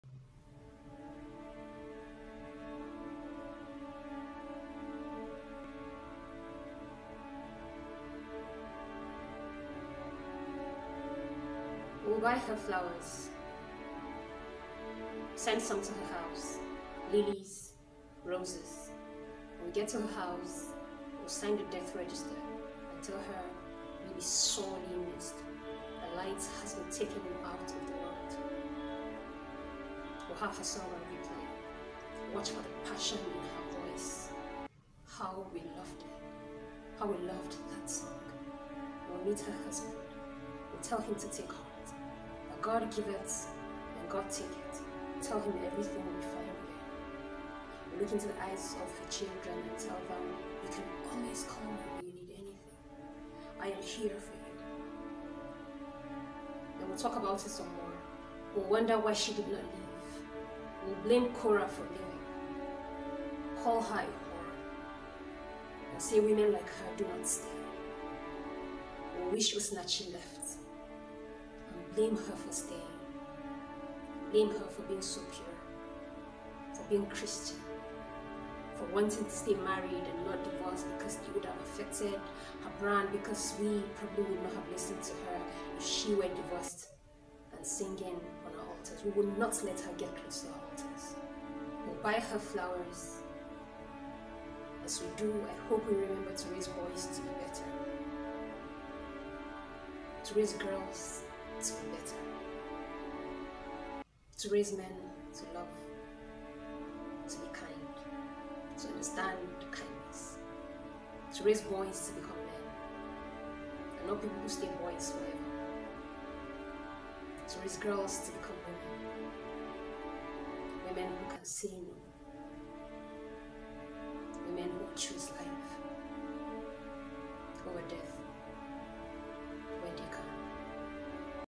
We Will Buy Her Flowers 🥀| Ekene May | Spoken Word Poetry
In light of the unfortunate passing of talented artiste, Osinachi Nwachukwu, it has become imperative for us to take parenting to whole new level.
Let's teach boys and girls right. Let's boys kindness, teach them to be men and girls to say no and to choose life over death, when they can.